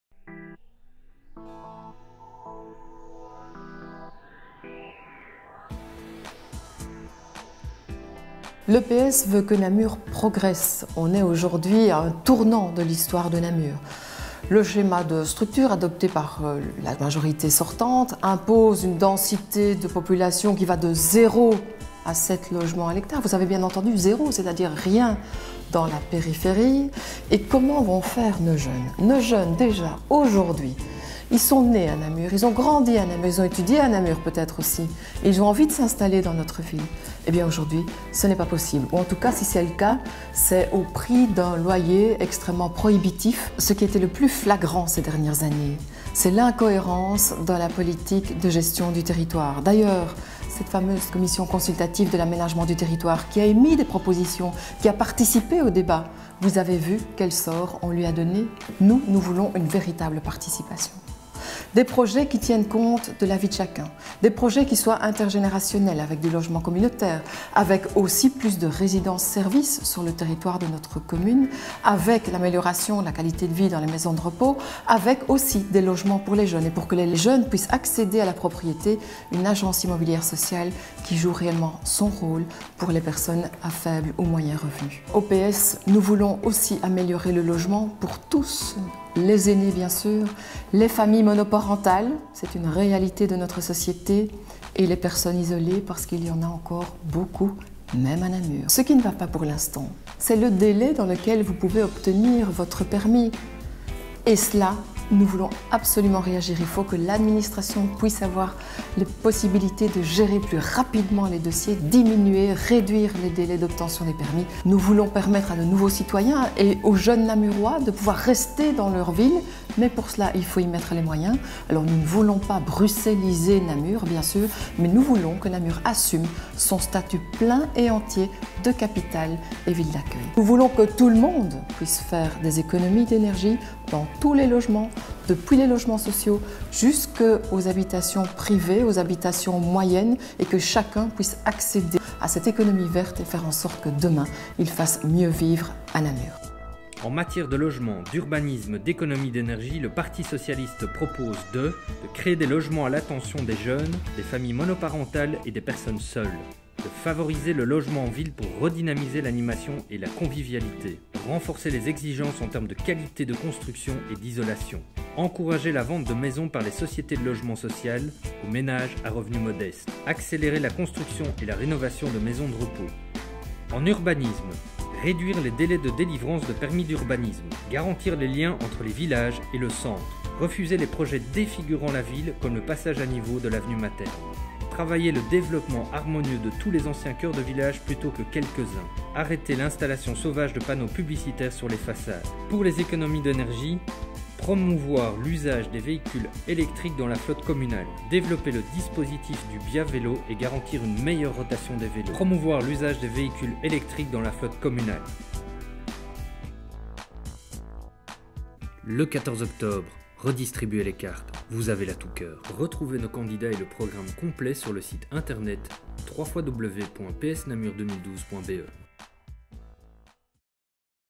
Namur 2012 - Au coeur du Logement: "Permettre aux jeunes de rester à Namur"
Eliane TILLIEUX: "Nous, au parti socialiste, nous voulons en terme de logement des projets qui tiennent compte de la vie de chacun. Des projets intergénérationnels, avec des logements communautaires, avec aussi plus de résidences services, et des maisons de repos avec une meilleure qualité de vie. Mais nous voulons aussi offrir à nos jeunes la possibilité de rester dans la commune qui les a vu grandir, voire même étudier. Actuellement, ils sont encore trop nombreux à quitter Namur pour les communes limitrophes. C'est pourquoi, nous proposons notamment la création d'une agence immobilière sociale qui permettrait aux personnes à faibles et moyens revenus (dont les jeunes, mais aussi les aînés, les personnes isolées et les familles monoparentales) d'accéder à la propriété..."